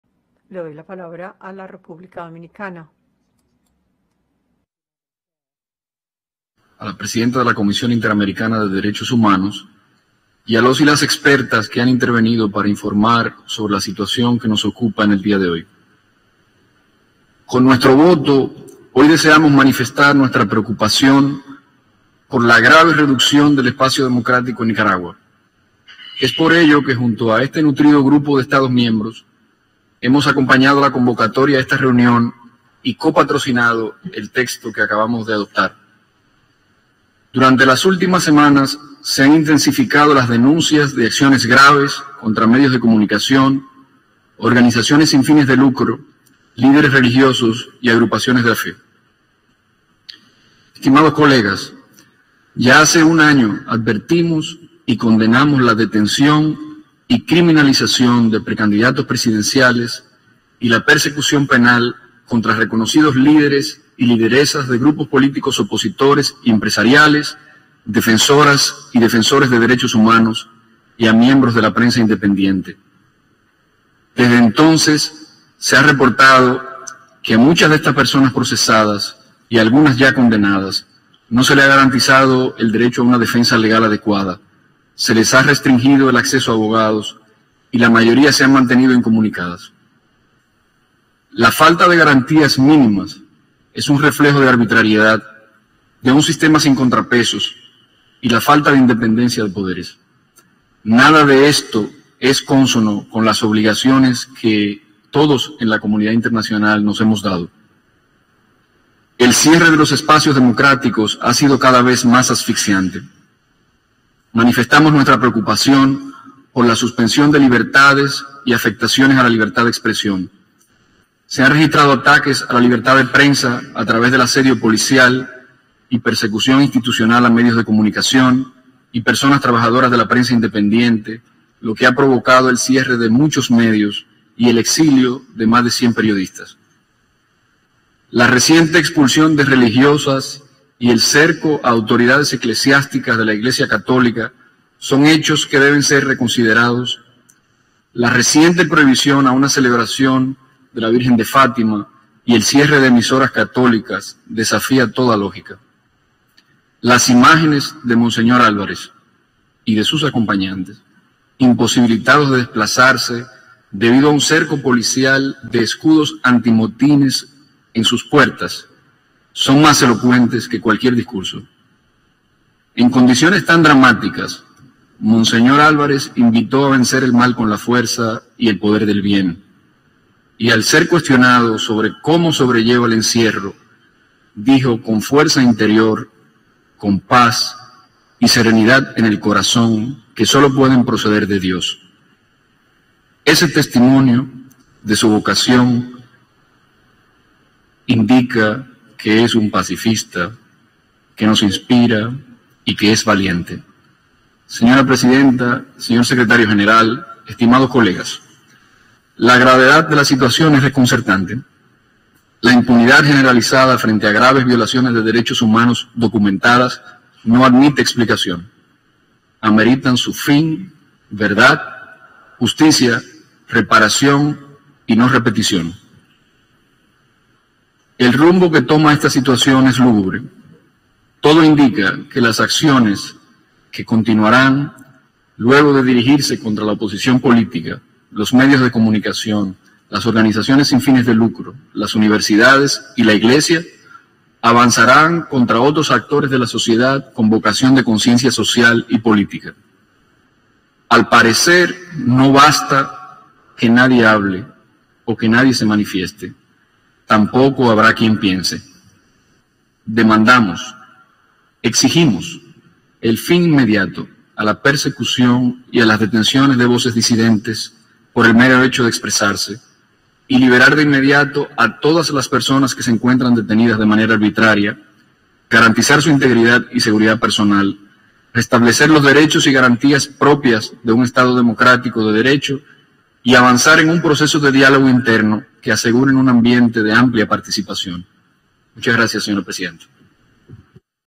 Intervención RD sobre la Situación en Nicaragua.
Intervención del Embajador Josué Fiallo Representante Permanente de República Dominicana ante la Organización de los Estados Americanos (OEA): “La situación en Nicaragua”.
Sesión extraordinaria del Consejo Permanente
12 de Agosto de 2022